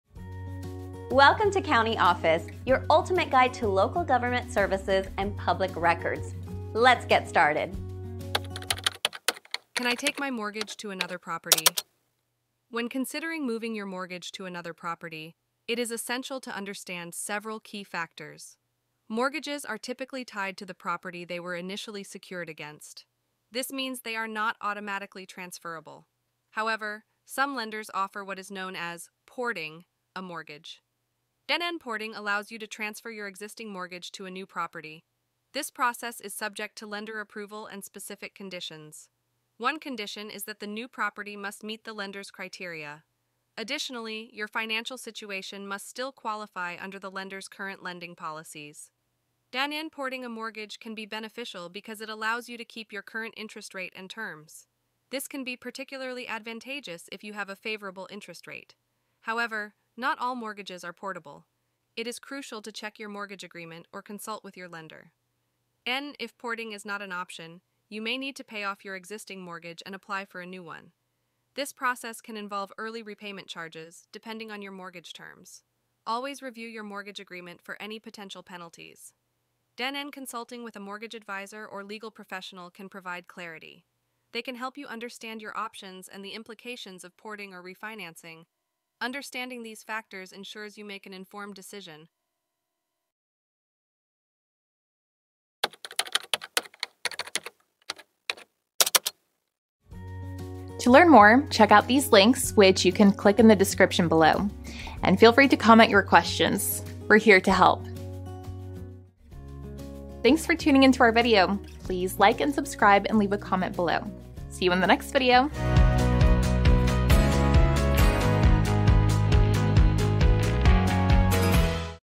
Can I Take My Mortgage To Another Property? - CountyOffice.org
Can I Take My Mortgage To Another Property? In this enlightening video, we delve into the intricacies of transferring your mortgage to another property. Mortgages are often associated with the property they were initially secured against, but some lenders offer the option to "port" your mortgage to a new property, subject to specific conditions and lender approval. Understanding the criteria, financial implications, and potential benefits of porting your mortgage is crucial. Find out how you can maintain your current interest rate and terms through porting, or explore the alternative of paying off your existing mortgage and applying for a new one. Always review your mortgage agreement for any potential penalties and consider seeking advice from a mortgage advisor or legal professional to make informed decisions regarding your mortgage transfer.